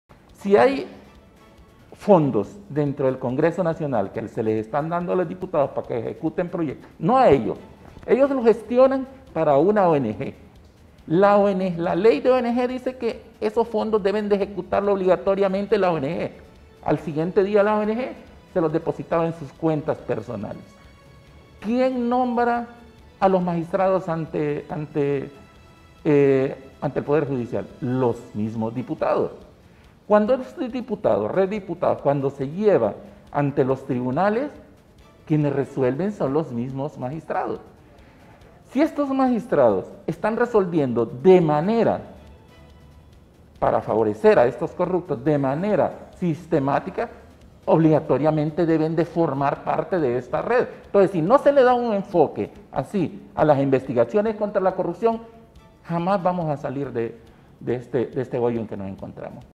UneNoticias | ForoUneTv 🔵
El jefe de la Unidad Fiscal Especializada Contra Redes de Corrupción (UFERCO), Luis Javier Santos, expresó que si hay fondos dentro del Congreso Nacional se les están dando a los diputados para que ejecuten proyectos.